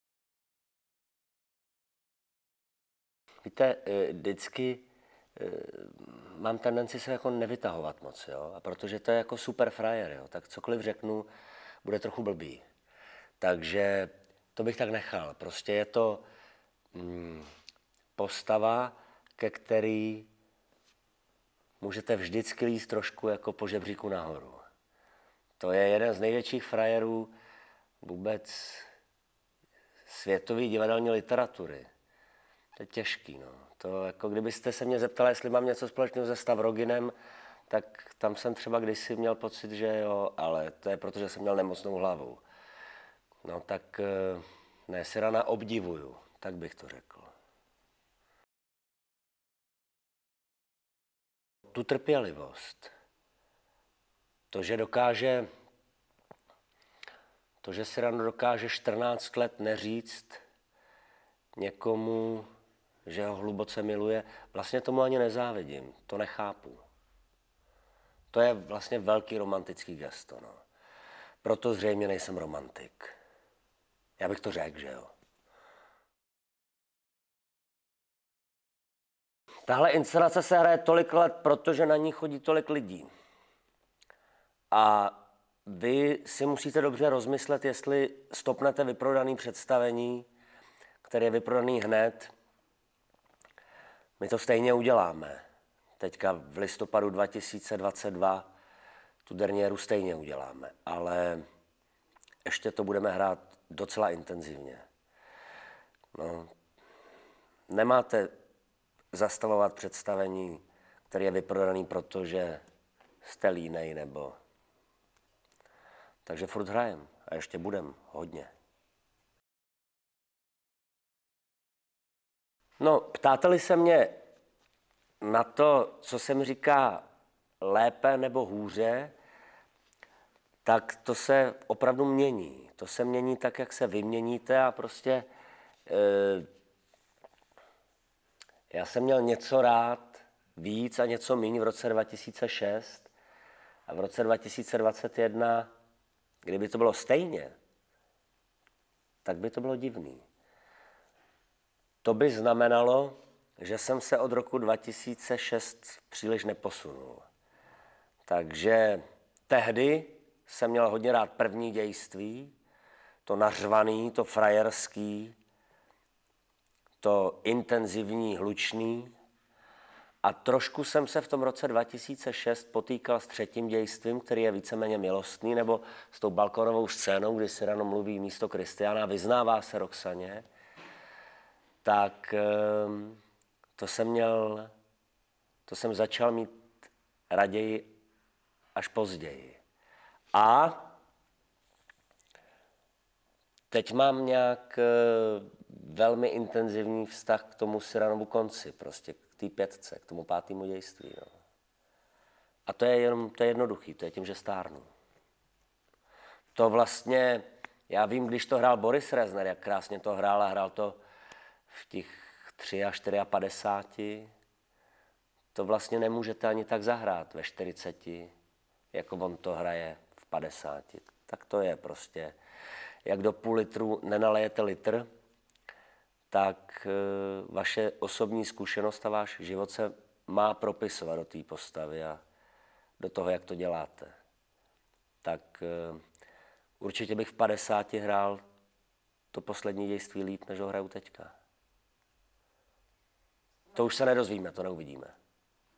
Martin Hofmann o Cyranovi | Dramox za oponou
🎥 Minulý měsíc jsme zavítali natočit Cyrana – legendu Divadla v Celetné.
 Martin Hofmann exceluje ve své životní roli jako nosatý šermíř z Bergeracu. Jeho Cyrano je fanfarónský, nedůtklivý a hrdý, ale také oddaně milující, zádumčivý a zraněný. Jeden z největších romantických příběhů všech dob hrají v Celetné již 15 let a představení je stále beznadějně vyprodané. Vychutnejte si dramatický příběh plný lásky, který nestárne, v nejvyšší kvalitě záznamu.

Ve videu se dozvíte více o inscenaci přímo od jejího režiséra Jakuba Špalka.